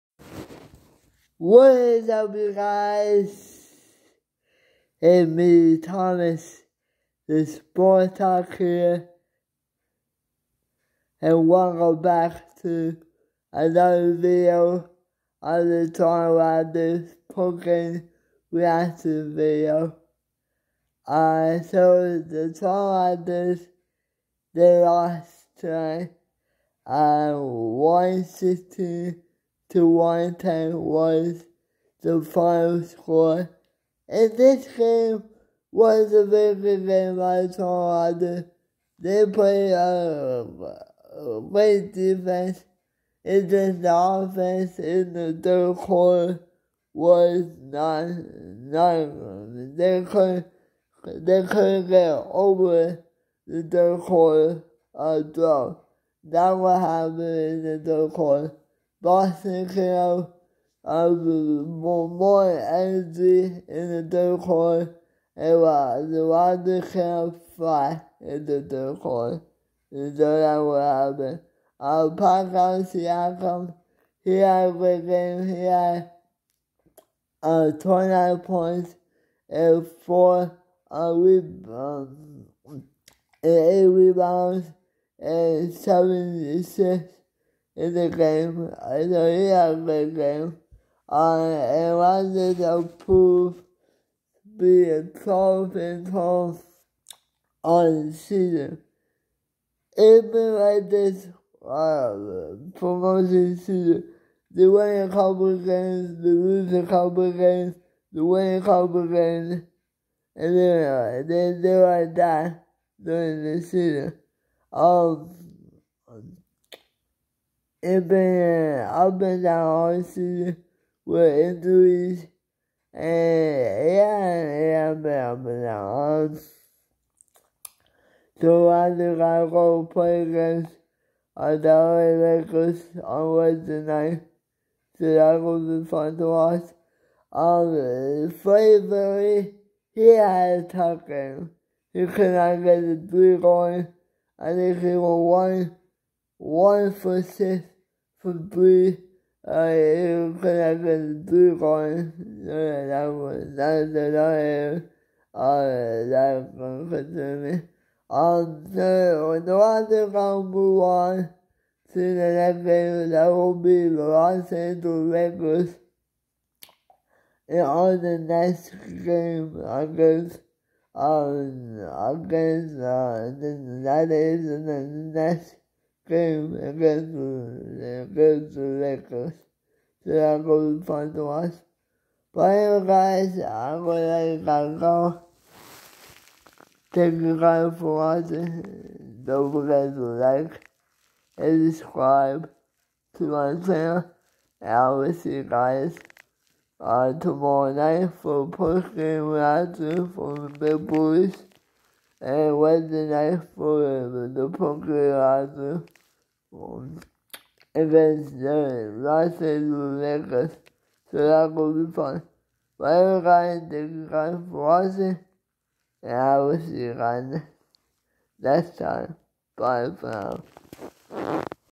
Raptors vs Celtics Game 24 Postgame Reaction
The Toronto Raptors have lost to the Boston Celtics 116-110. The Toronto Raptors drought in the 3rd quarter was the reason why the Toronto Raptors did not win. Pascal Siakam has scored 29 points in the game against the Boston Celtics. The Toronto Raptors will play against Los Angeles Lakers on Wednesday night.